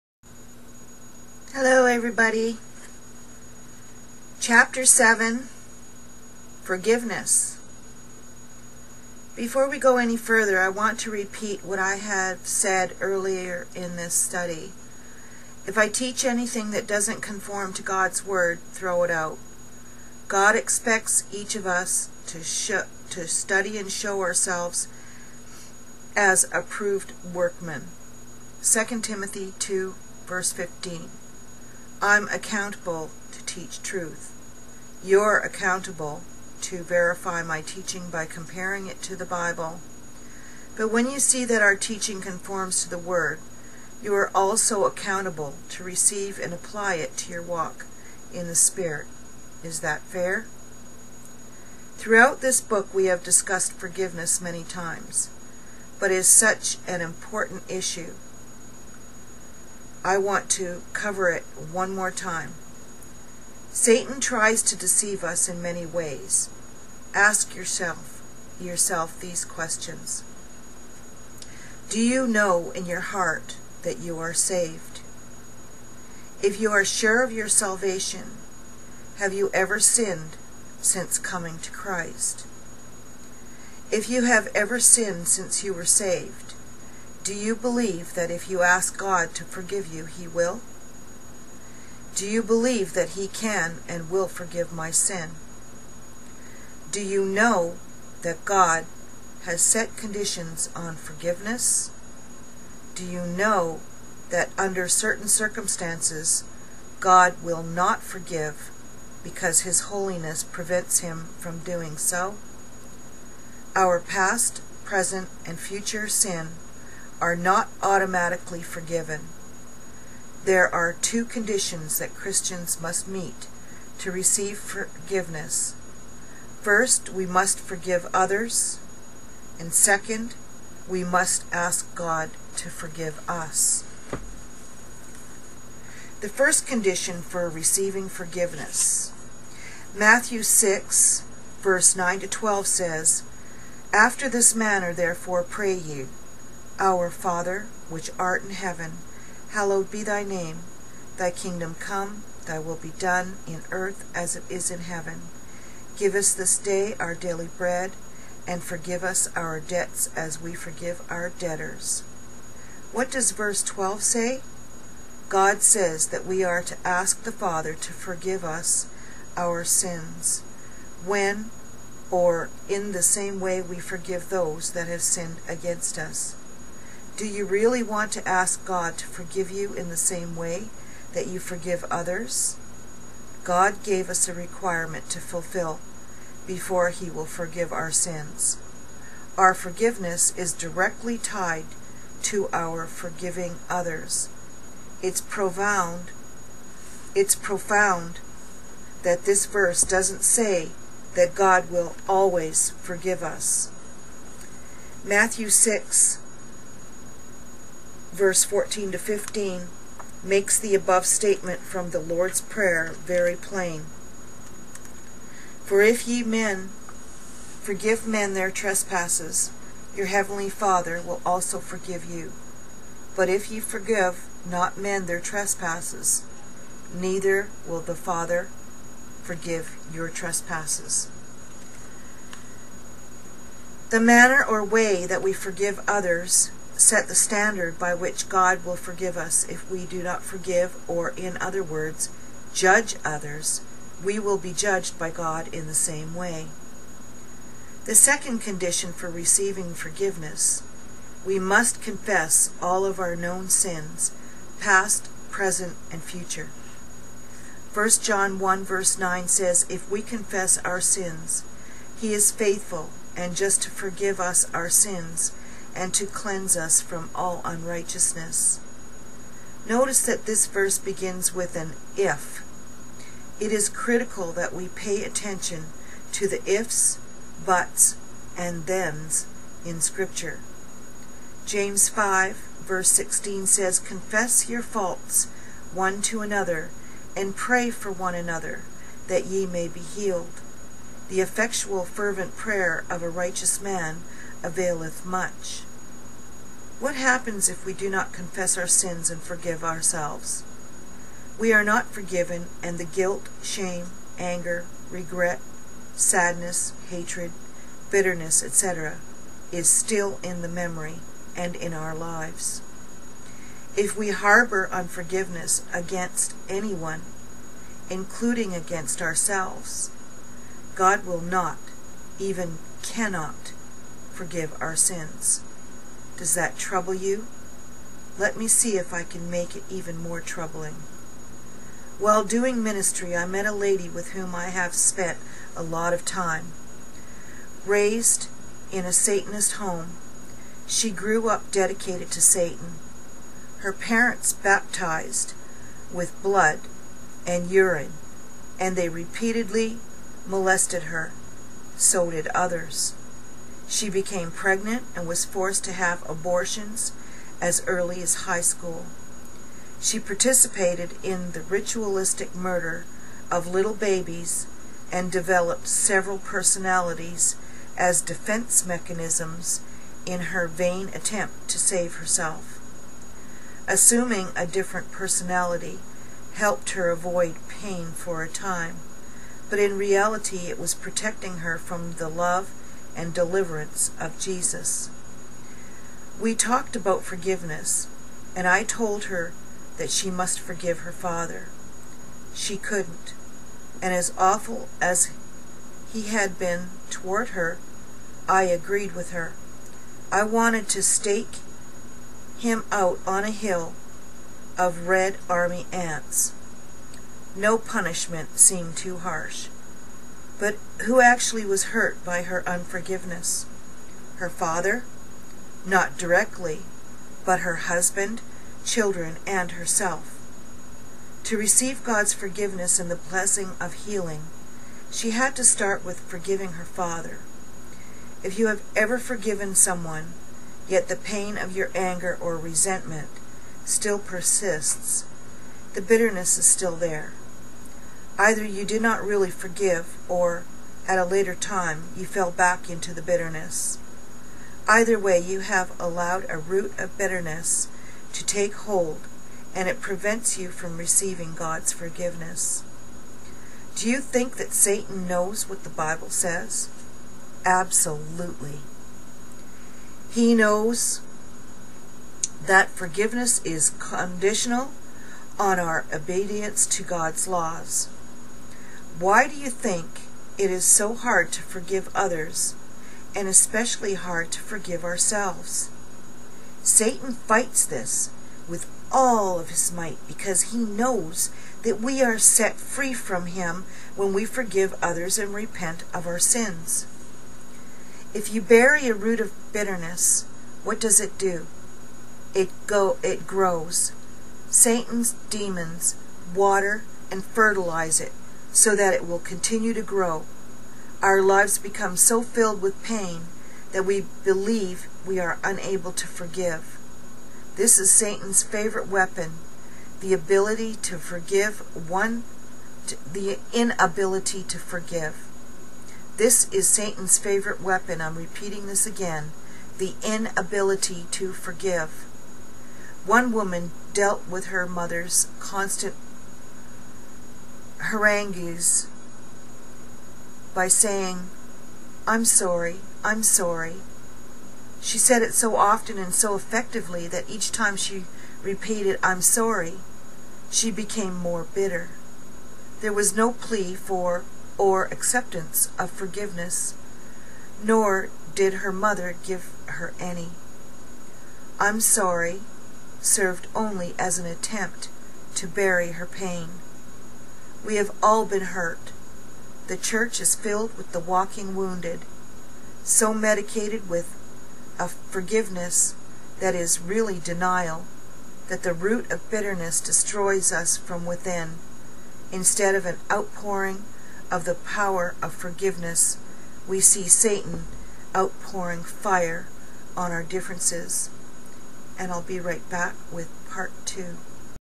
Forgiveness Part 1
The beginning of a new chapter 1 of 5 parts. Chapter 7.. if you are jsut starting to view this video for the first time then I suggest you listen but please go back to chapter 1 cause all roots of these accusing spirits start with one spirit. He accuses us day and night to Our most High God. Have you noticed the spirit of evil in the world. Bitterness, resentment, anger, hate etc in this world? This is why I am doing this to help people understand why they might be feeling this way. Replace satans lies with God's Truth today.